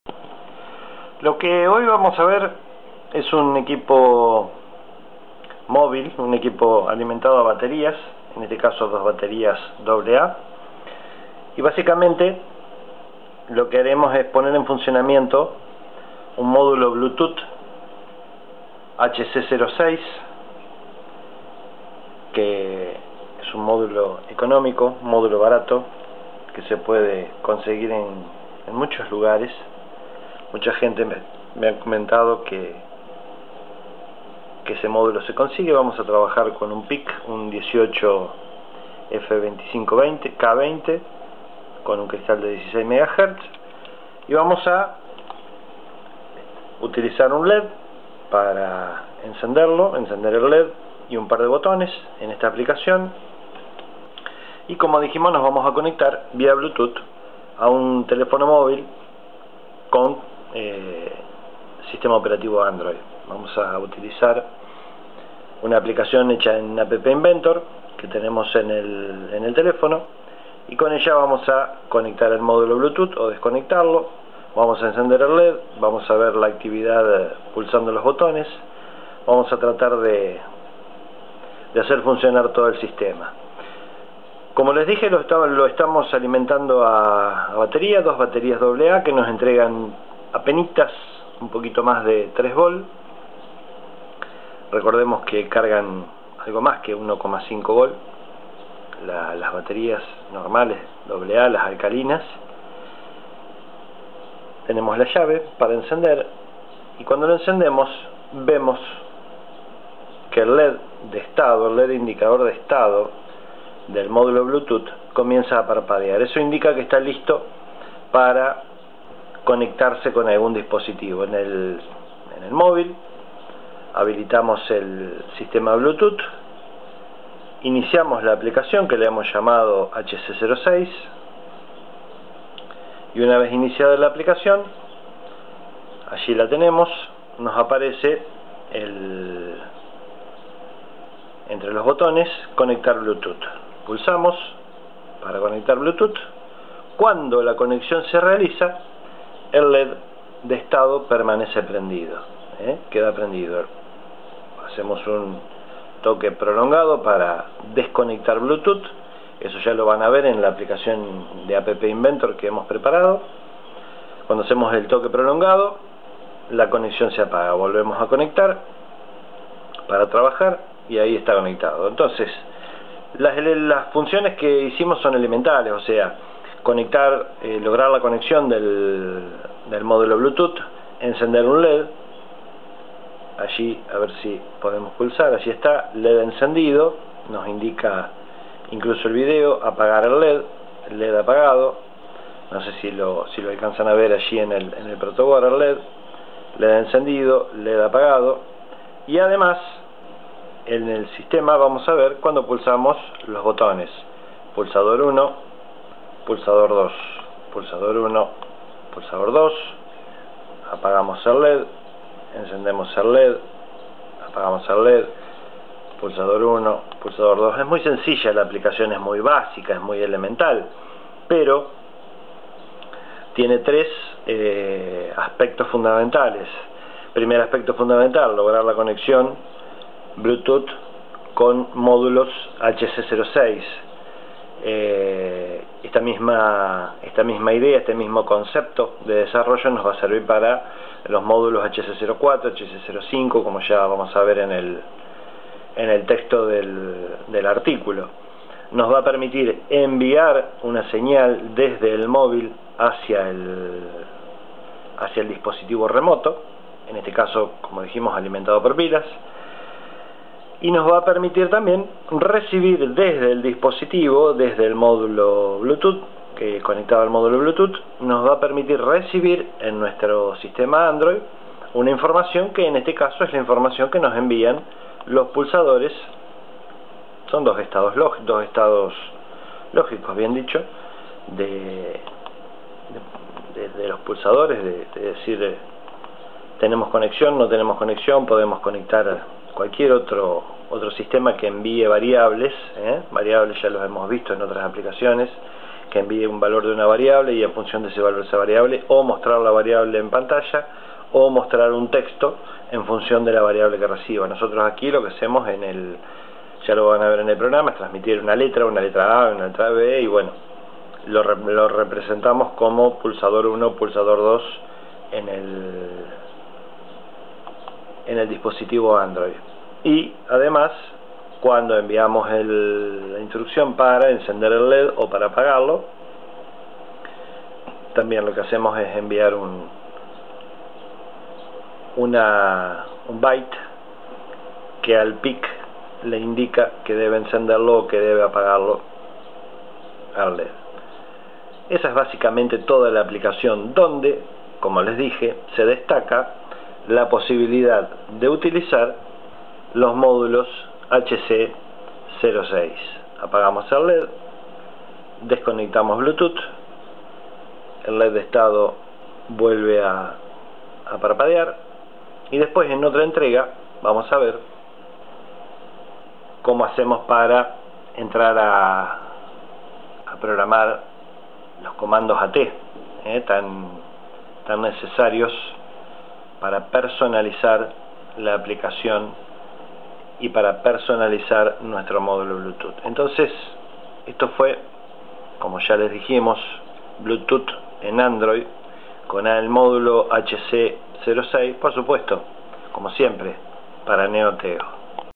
Bluetooth_HC-06_en Android.avi
Como utilizar el módulo Bluetooth HC-06 en una aplicación Android. Ante la excasa y confusa información que existe en la web sobre estos económicos módulos, nos lanzamos a ensayar con ellos para descubrir su funcionamiento y poder aprovechar así uno de los módulos más económicos del mercado. Con velocidades que superan el Megabaud por segundo y un tamaño muy pequeño, estos módulos pueden convertirse en tu próxima selección para futuras aplicaciones.